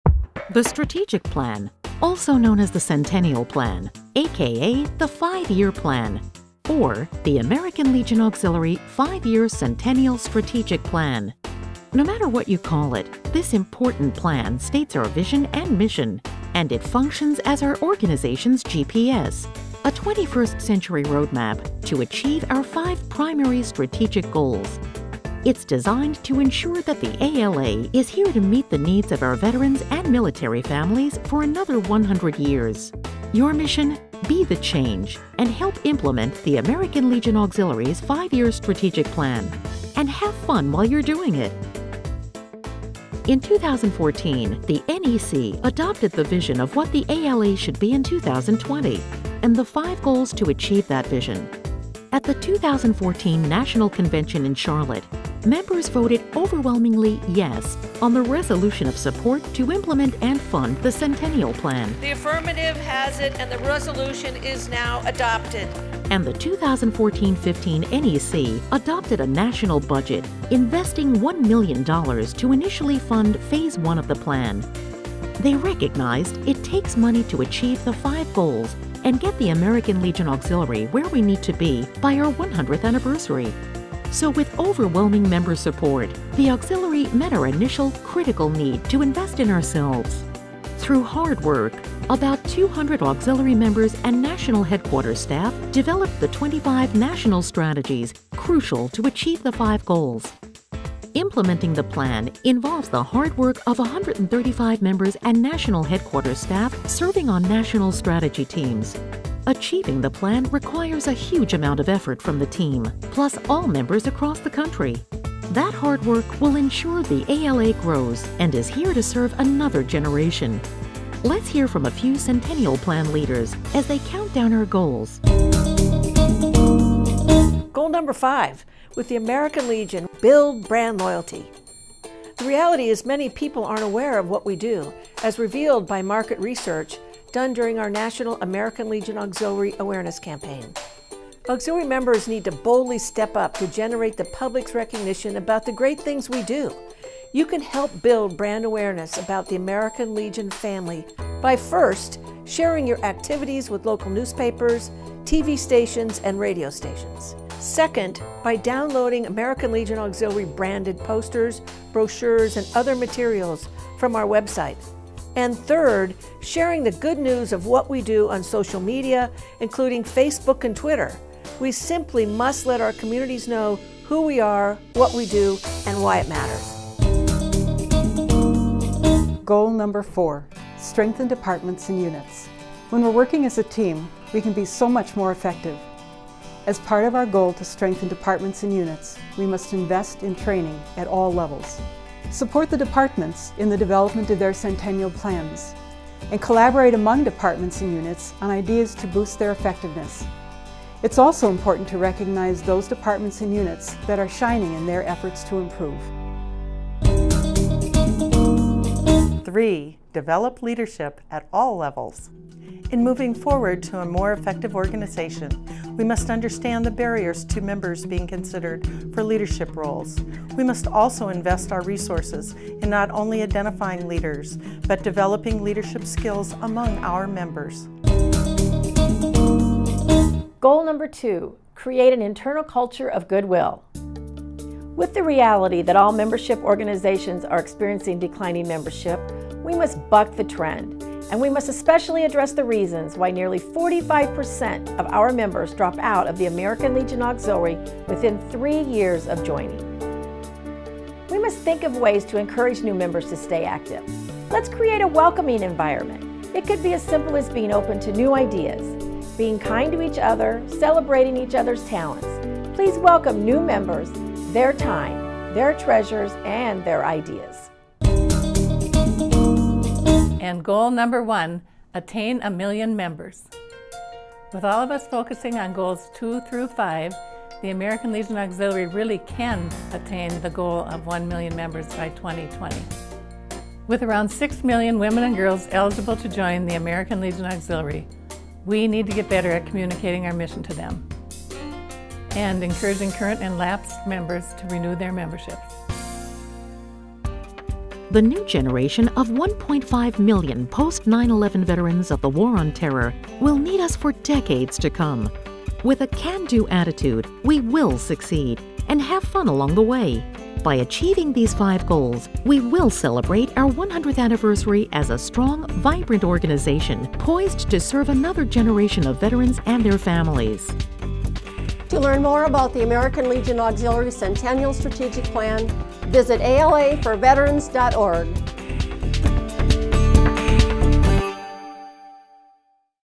Centennial Strategic Plan | American Legion Auxiliary
The Strategic Plan…also known as the Centennial Plan…aka The 5-Year Plan…or, The American Legion Auxiliary 5-Year Centennial Strategic Plan. No matter what you call it, this important plan states our vision and mission. And, it functions as our organization’s roadmap to achieve our five primary strategic goals. It’s our plan to ensure we’ll meet the needs of our veterans and military families for another 100 years. Your mission: Help implement the American Legion Auxiliary’s 5-Year Strategic Plan -- and have fun while you’re doing it!

For more information about the American Legion Auxiliary's ongoing support of veterans, military, and their families, please

Produced by Highpoint Productions